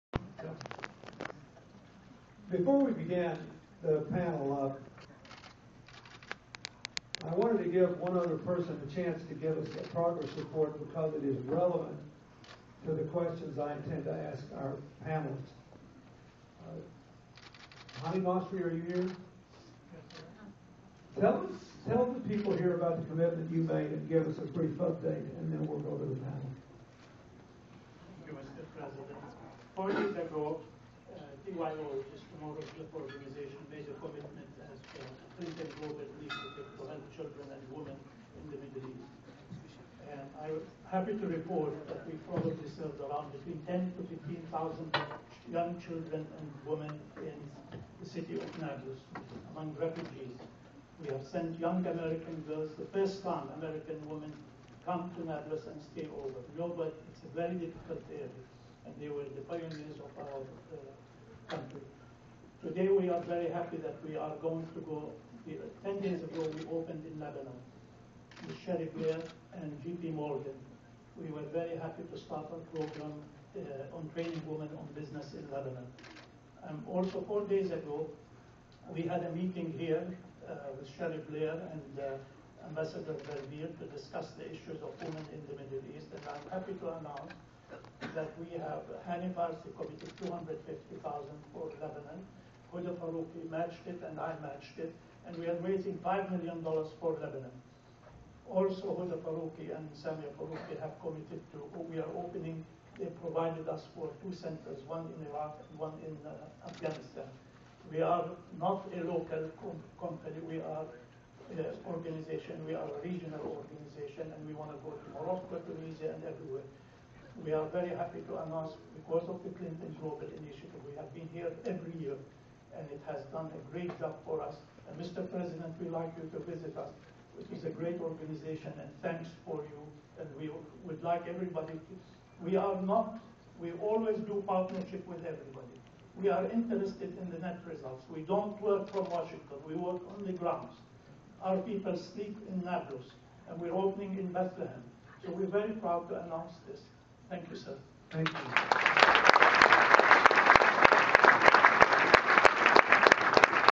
President Clinton and Hani Masri at the 2011 CGI special panel on the Middle East
President Bill Clinton introduces Hani Masri at the 2011 Clinton Global Initiative to give an update on TYO's commitments before the special panel on the Middle East